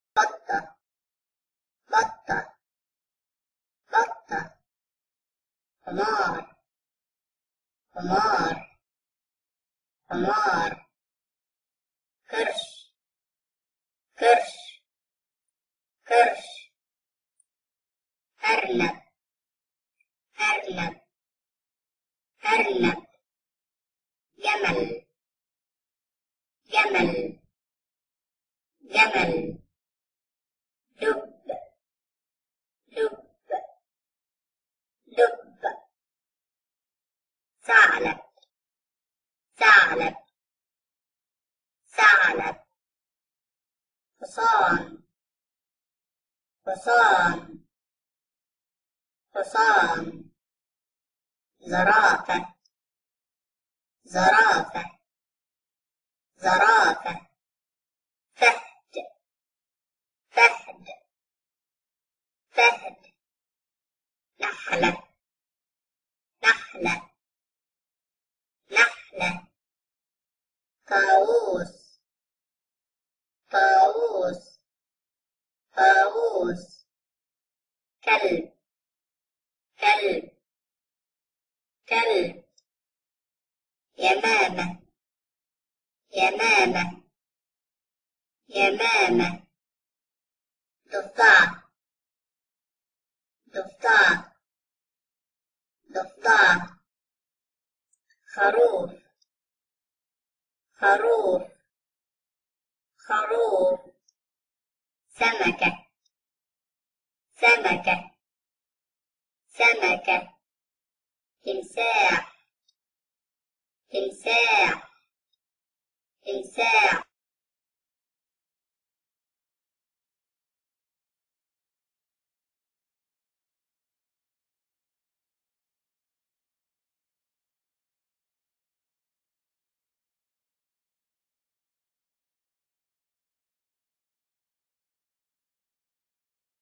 تعليم اسماء الحيوانات للطفل باللغه العربيه باسلوب جميل - كل شيء مفيد
يتضمن الفيديو محتوي شيق من انتاج قناة كل شيء مفيد يهدف الي زيادة قدرة الطفل علي النطق وتعلم اسماء الاحيونات والاشياء الاخر والالوان ومسعادة الطفل اكتشاف العالم من حوله,
وذلك باسلوب شيق وجميل يجزب الطفل ويسليه ويسعده علي التعلم بسهوله
قناة كل شيء مفيد تقدم لكم فديوهات حصريه من انتاجهاوتهدف الي نشر العلم للكبير والصغير طيور الجنة طيور الجنة